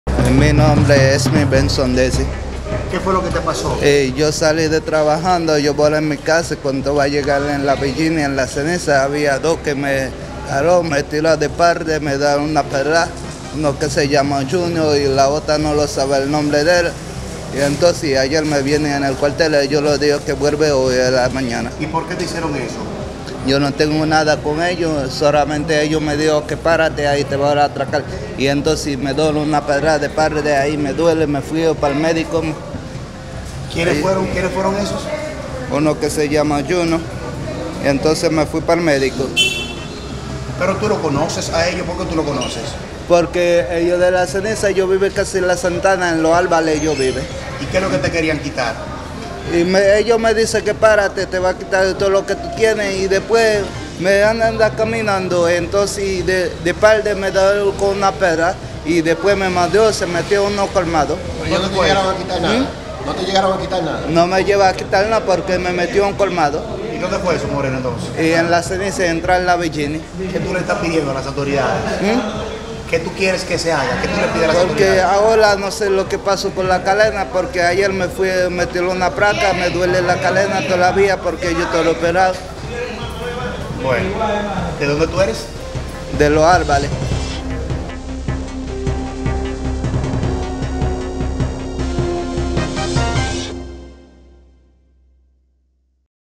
Extranjero denuncia le propinaron una pedrada en Las Cenizas SFM
San Francisco de Macorís.- Un joven denunció el lunes 17 de junio, haber sido víctima de un intento de atraco por parte de dos individuos en Las Cenizas.

El denunciante relató que los agresores, a quienes conoce porque residen en la misma zona, lo interceptaron y le exigieron que entregara sus pertenencias, sin embargo, el joven logró escapar y refugiarse en un colmado cercano.

A pesar de no ser despojado de sus pertenencias, el joven sufrió varias pedradas durante el incidente, por lo que tuvo que recibir atención médica.